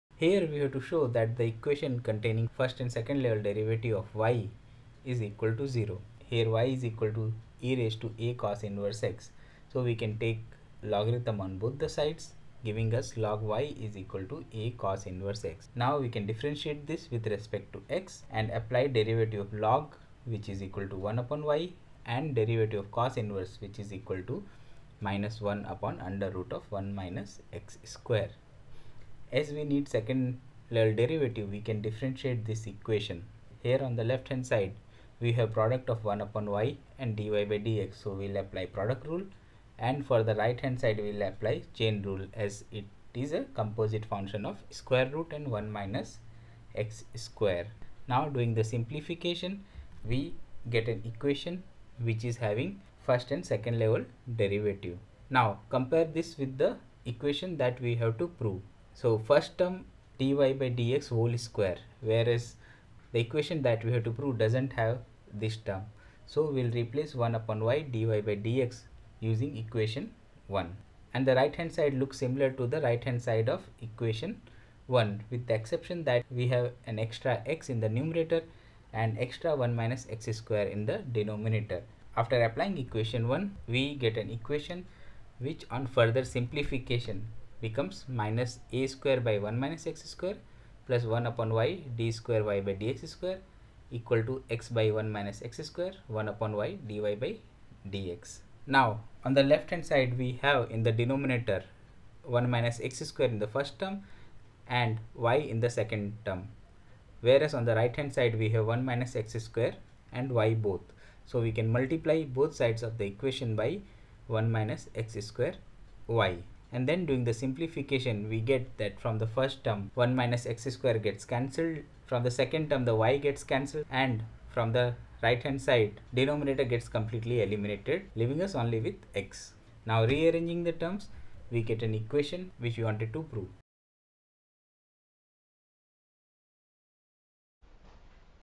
If y = e^(a cos^-1 x), show that (1- x^2) d^2y/dx^2 - x dy/dx - a^2y = 0
CM12.5.M.22 CBSE class 12 Maths Chapter 5 Continuity and differentiability Miscellaneous Exercise Question 22 solution

00:00 If y = e^{a cos^-1 x}, show that (1- x^2) d^2y/dx^2 - x dy/dx - a^2y = 0